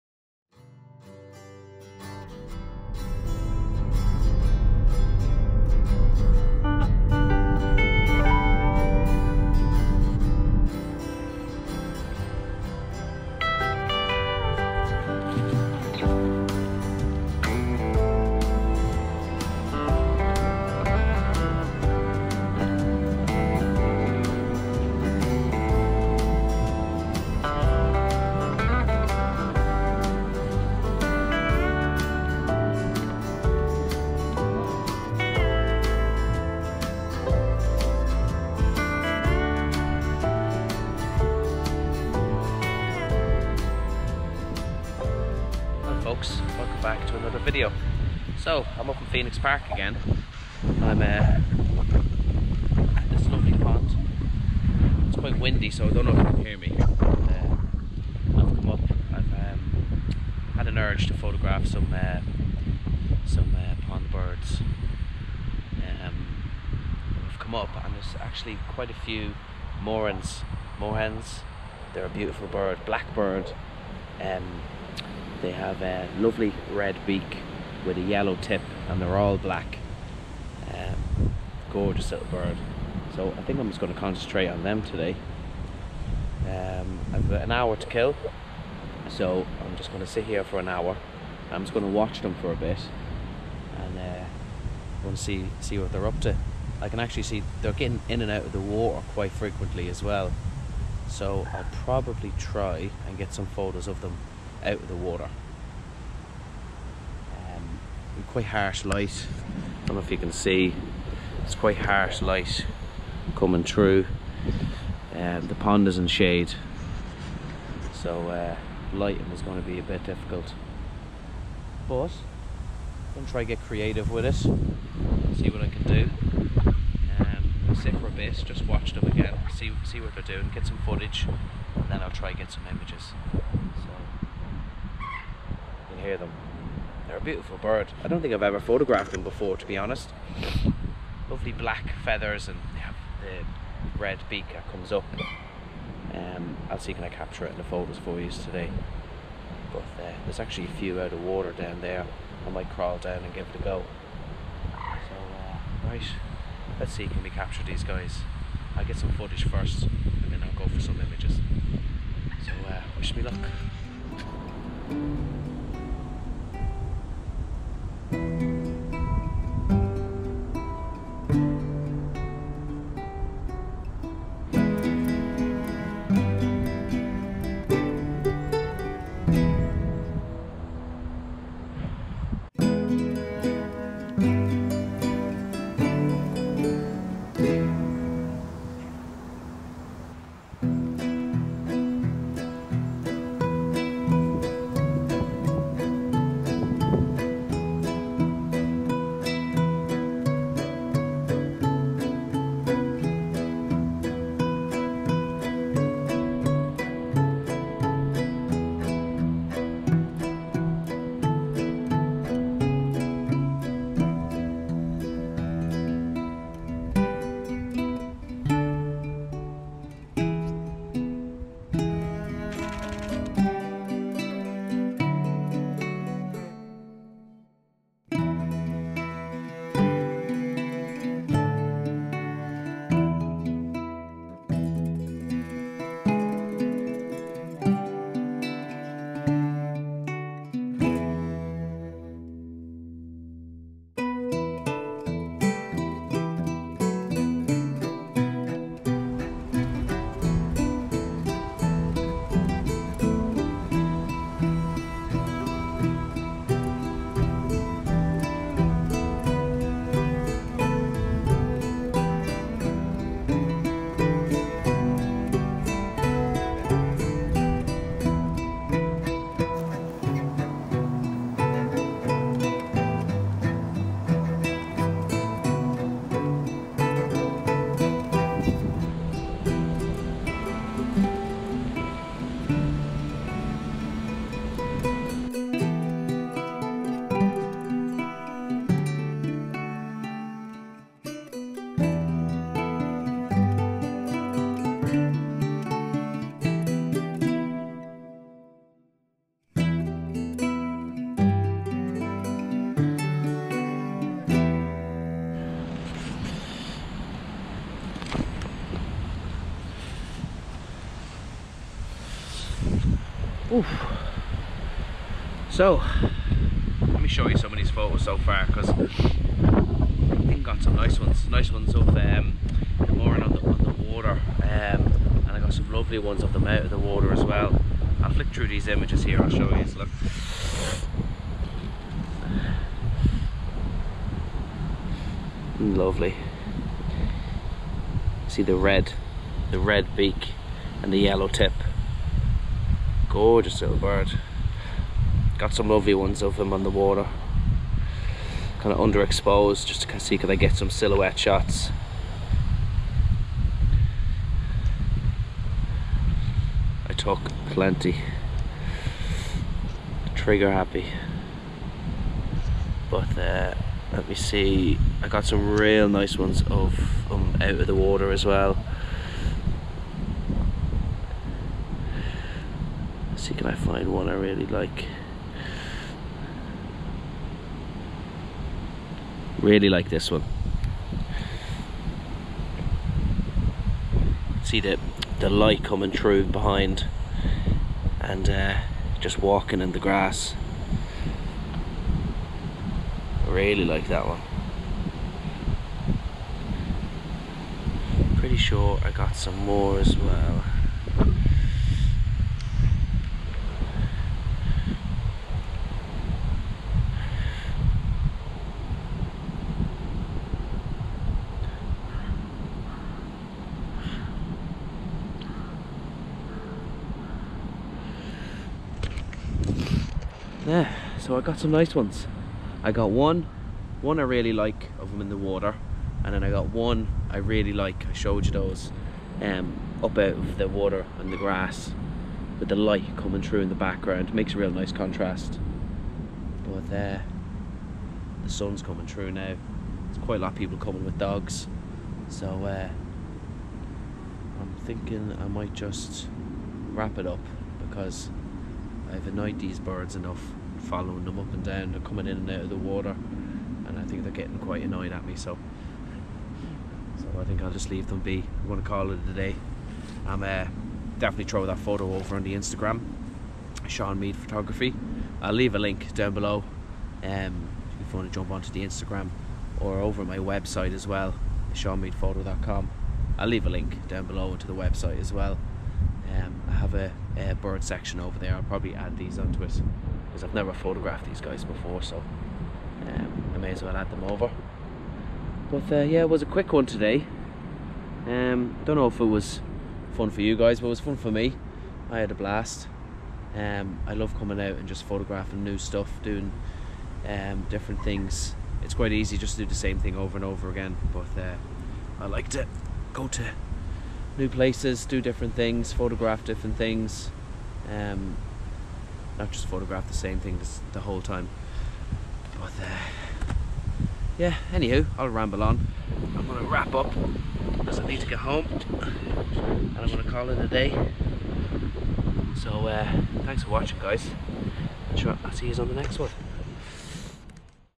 Bird Photography - Water Birds (Moorhen) Nikon Z6ii
Lovely morning out watching and photographing pond birds. Not the best images but always nice to get out with the camera.

Images taken with the Sigma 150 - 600mm (C) along with the Nikon Z6 ii.
Vlogging with the GoPro Hero 7 Black